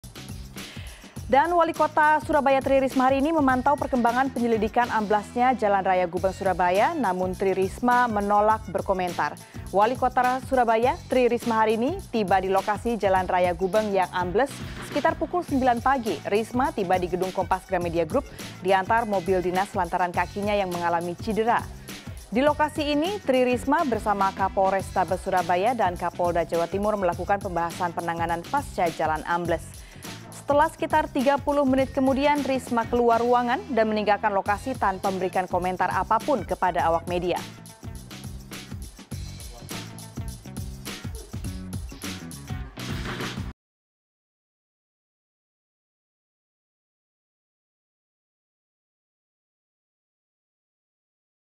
Risma Selidiki Jalan Gubeng Amblas
Walikota Surabaya Tri Rismaharini Bersama Kapolda jatim dan kapolrestabes surabaya, hari ini, datangi jalan raya gubeng yang ambles. Setelah 30 menit, risma meninggalkan lokasi, tanpa komentar.

Like & Follow Facebook:
CNN Indonesia

cnnindonesiatv